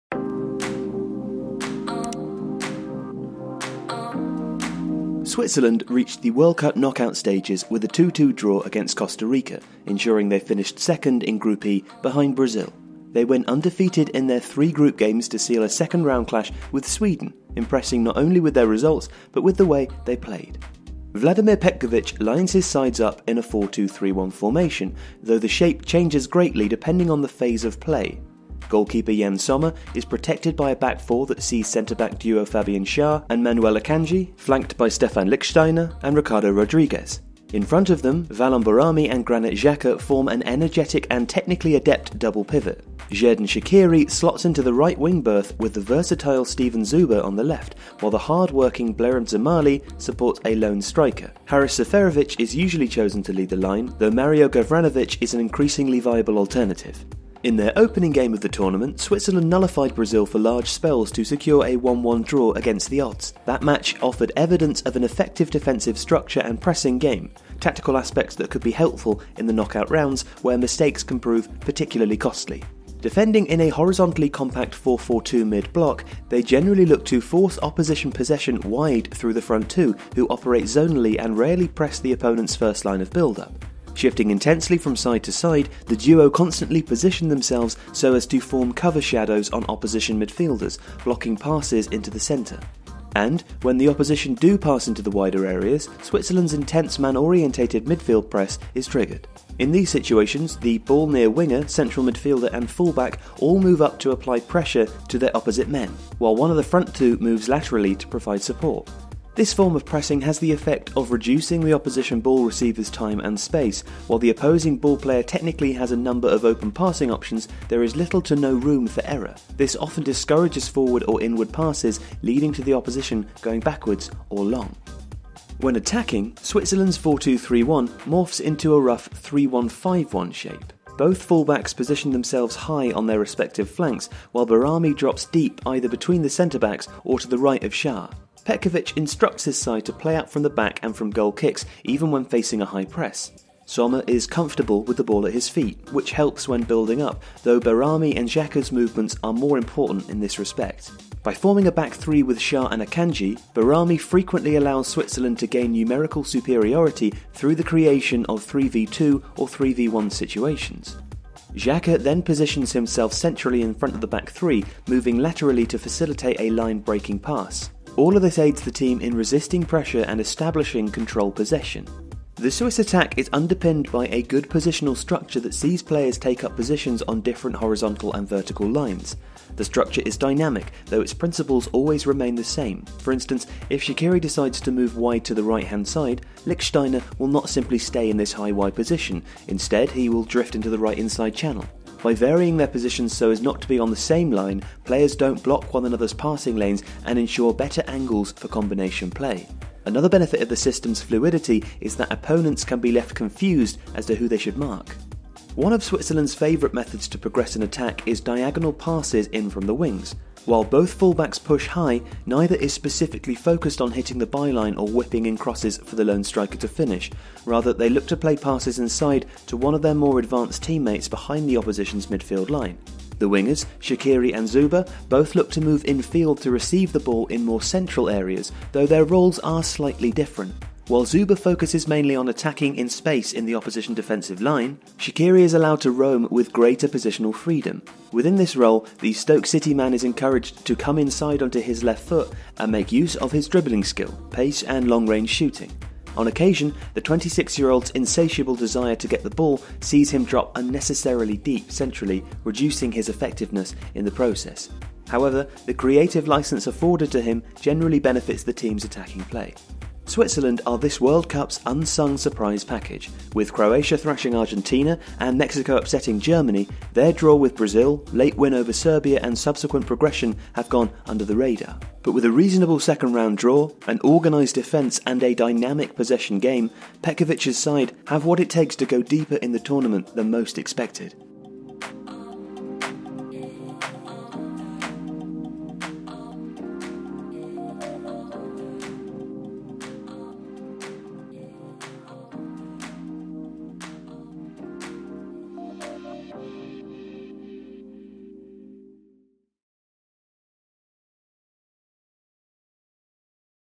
FIFA World Cup 2018™: How Switzerland Progressed
Switzerland reached the World Cup knockout stages with a 2-2 draw against Costa Rica ensuring they finished second in Group E behind Brazil. They went undefeated in their three group games to seal a second round clash with Sweden, impressing not only with their results, but with the way they played.

and follow Tifo Football elsewhere

Produced by Tifo Studios: Taking an illustrated look into the beautiful game. Official Media Partner of the Bundesliga.